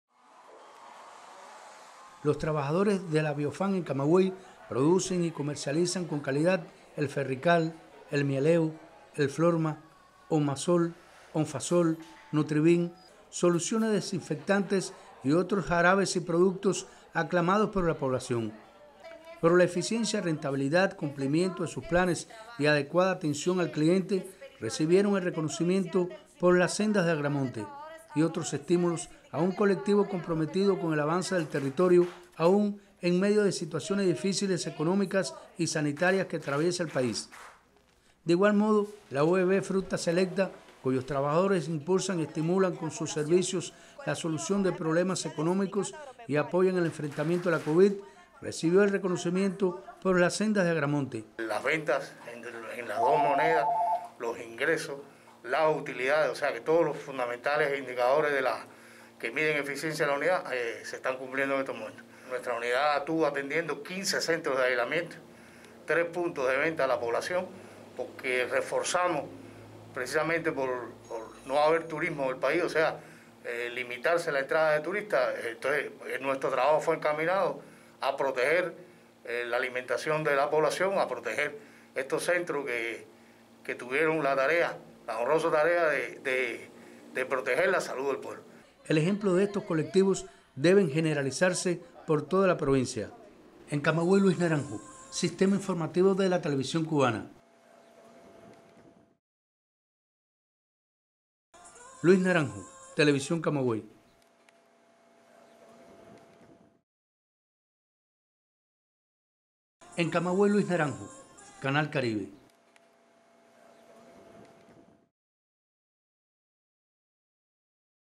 Continua en Camagüey el reconocimiento a trabajadores por su destacado desempeño ante la Covid-19
Varias entidades en Camagüey han recibido el reconocimiento Por las Sendas de Agramonte, que entrega la Central de Trabajadores de Cuba en esta provincia para resaltar la labor de enfrentamiento a la Covid-19. Entre esos colectivos sobresalen las empresas Labiofam y Fruta Selecta, y de esta última comenta Vladimir Arnaez, director de esa unidad empresarial de base.